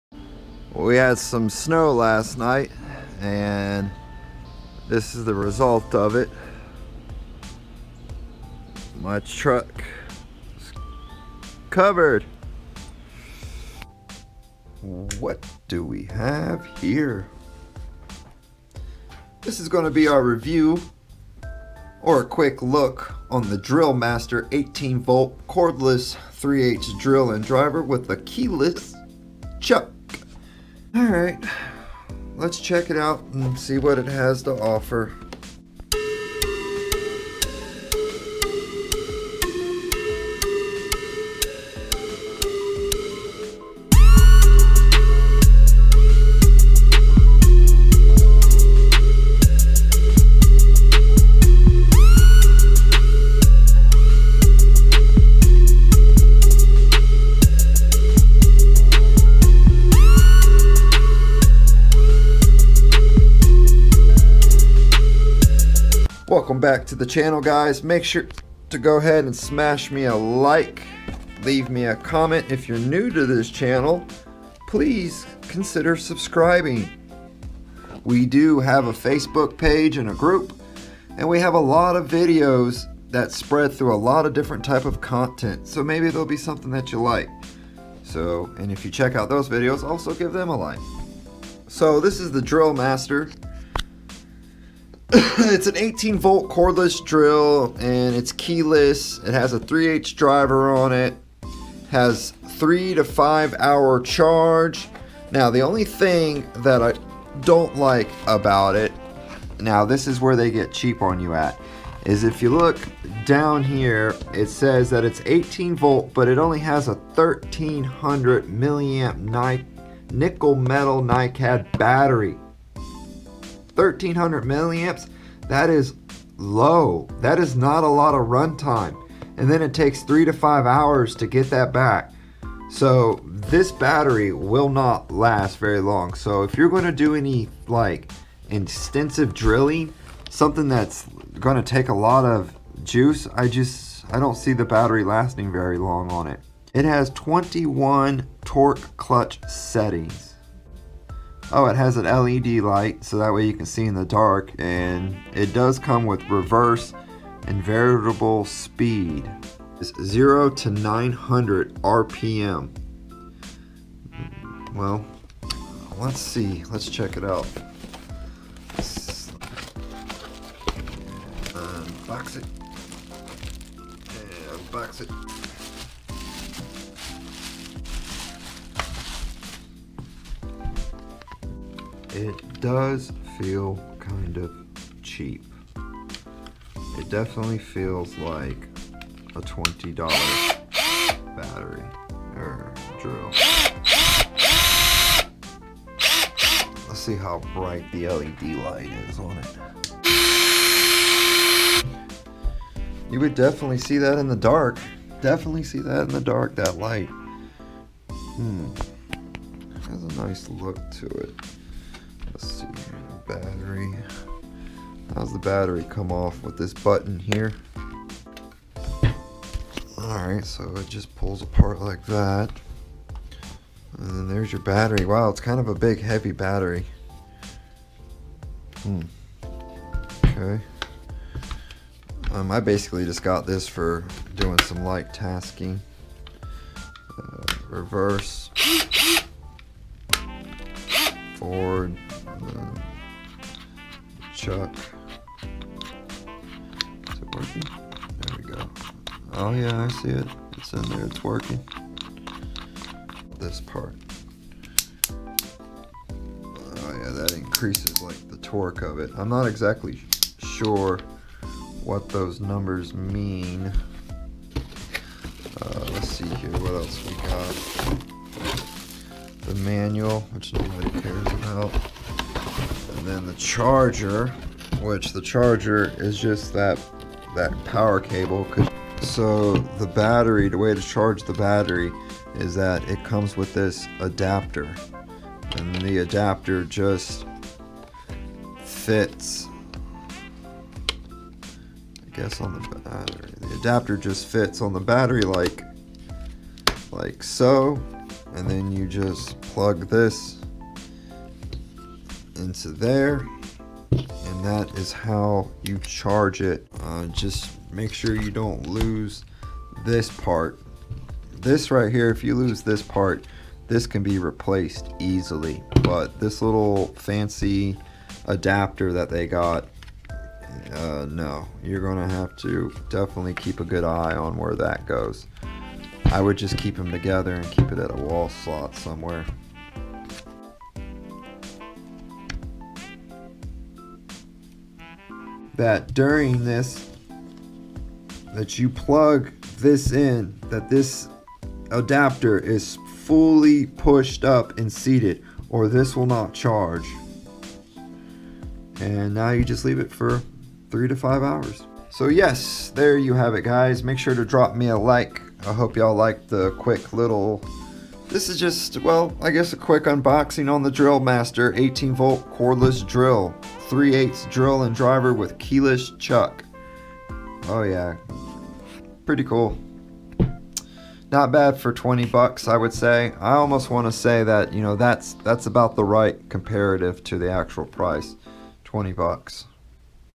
Drill Master 18 Volt Cordless 3/8" Drill with Keyless Chuck /✅unboxing✅
need a 20$ drill ? want one that's cordless? well drill master has just the drill for you .. lets unbox it and take a quick look at it... please subscribe .. smash a like and Leave a commentSUBSCRIBE & LIKE⚡️ ⚡️ LINKS BELOW ⚡️